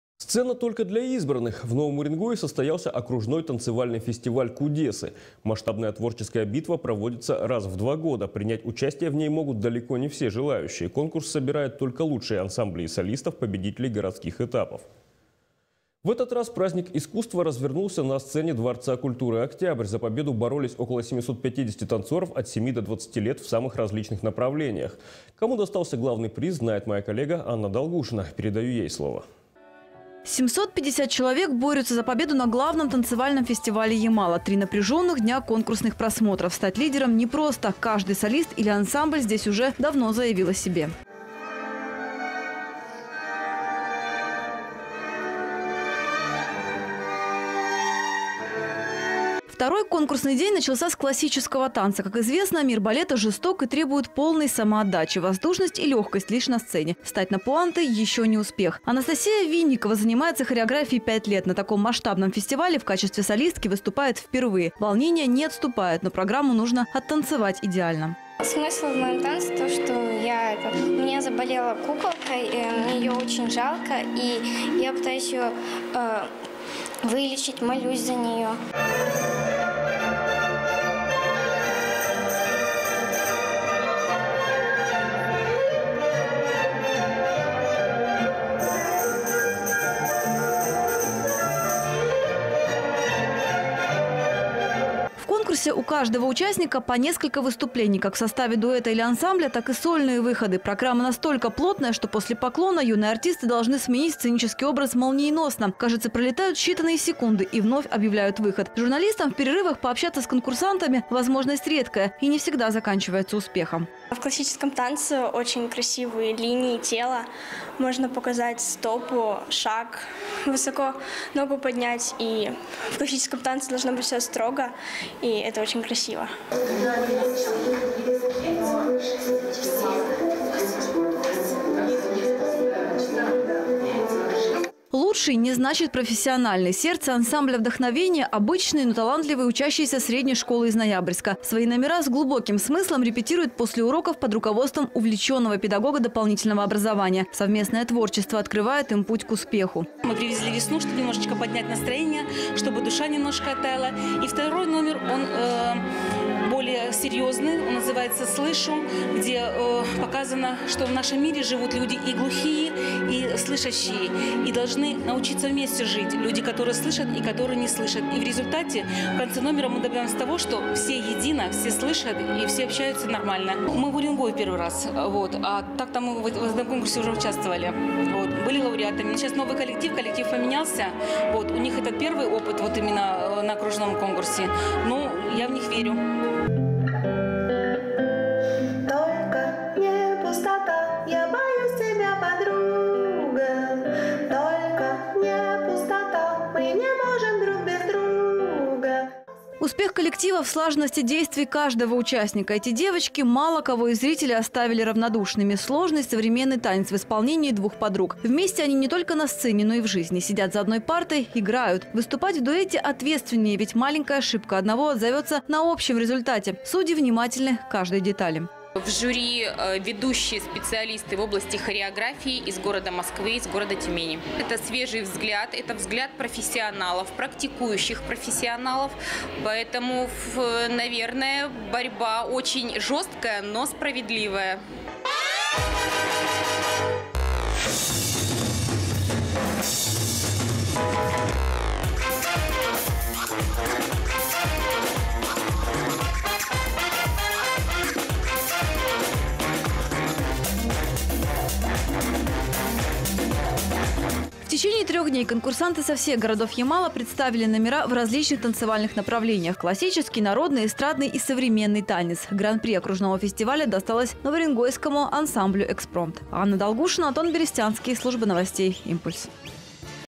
Гран-при окружного фестиваля танца «Кудесы» остался в Новом Уренгое
цена только для избранных. В Новом Уренгое состоялся окружной танцевальный фестиваль «Кудесы». Масштабная творческая битва проводится раз в два года. Принять участие в ней могут далеко не все желающие. Конкурс собирает только лучшие ансамбли и солистов – победителей городских этапов. В этот раз праздник искусства развернулся на сцене дворца культуры «Октябрь». За победу боролись около 750 танцоров от 7 до 20 лет в самых различных направлениях.